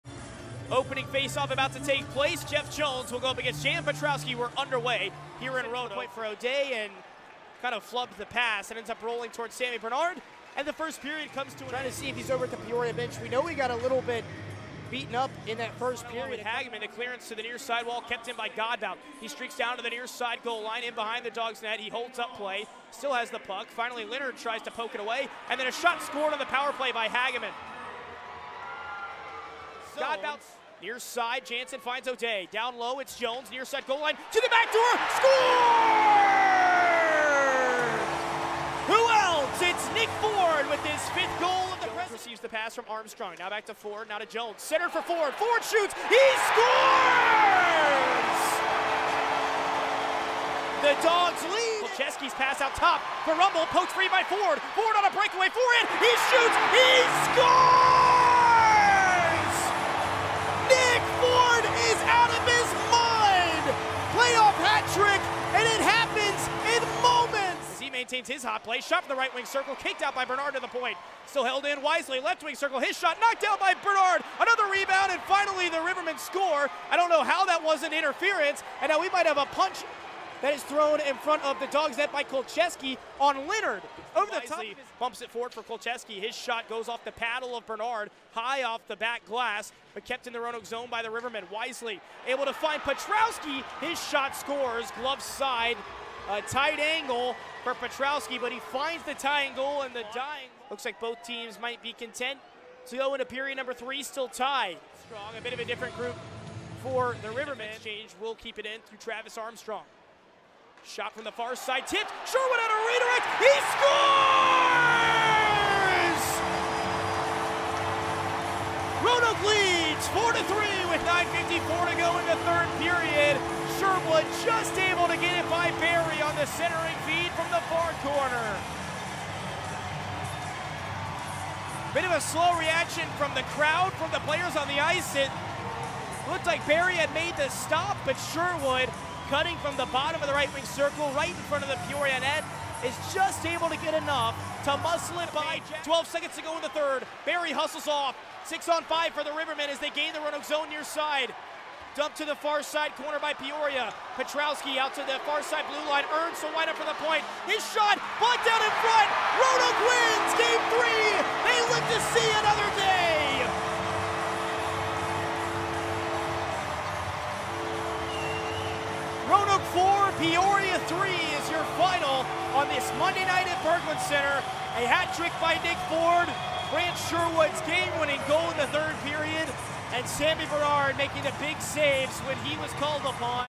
Presidents Cup Finals - Game Three - Peoria Rivermen 3 @ Roanoke Rail Yard Dawgs 4
Nick Ford Leads Roanoke to a game 4